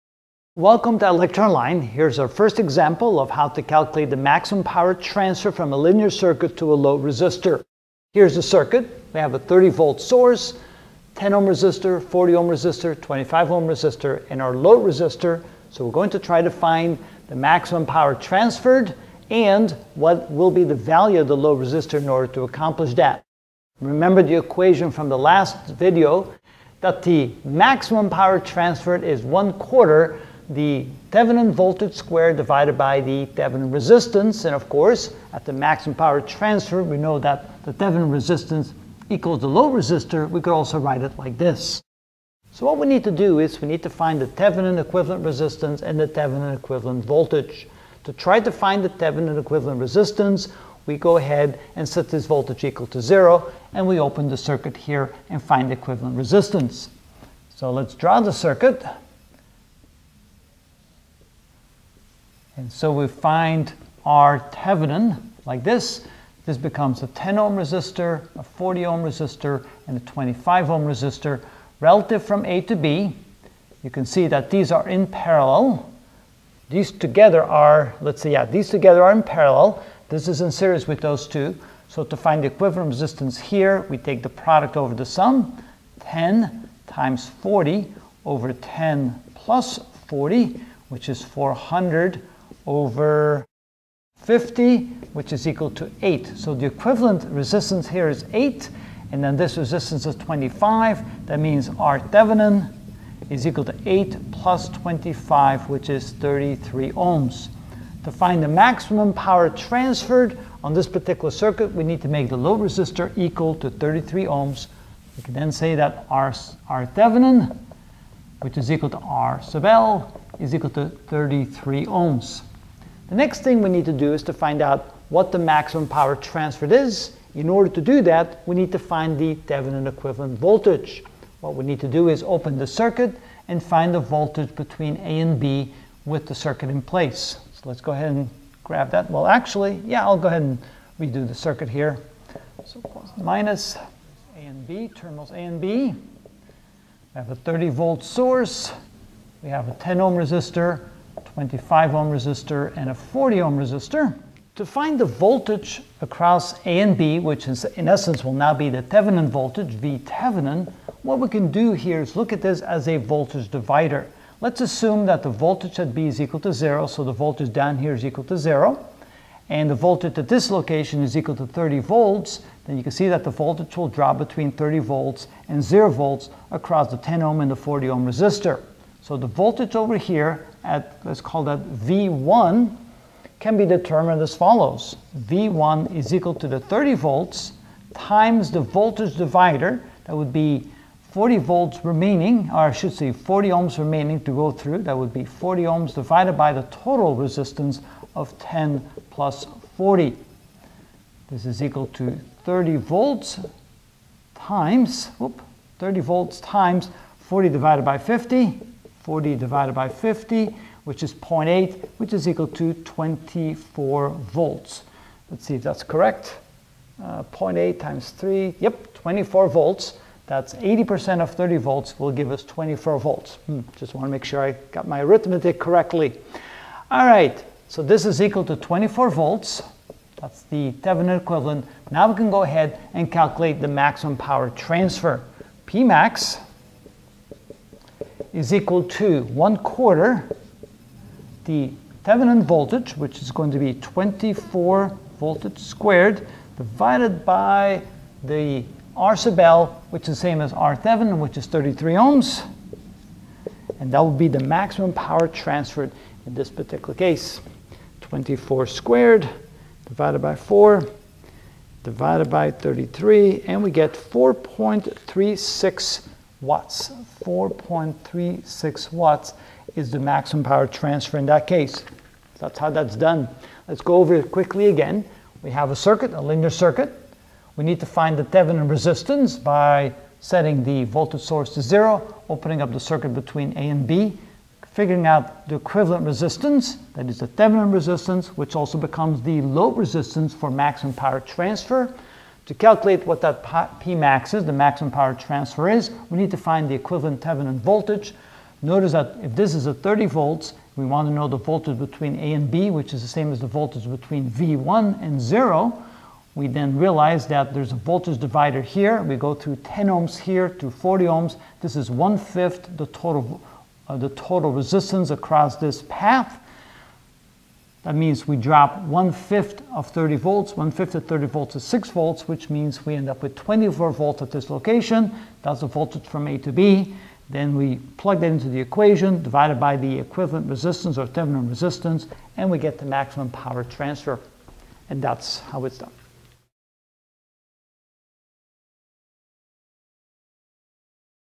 Electrical Engineering: Ch 4: Circuit Theorems (28 of 35) Maximum Power Transfer Ex. 1
In this video I will find P(max)=?  the maximum power transfer of the linear circuit to a load resistor.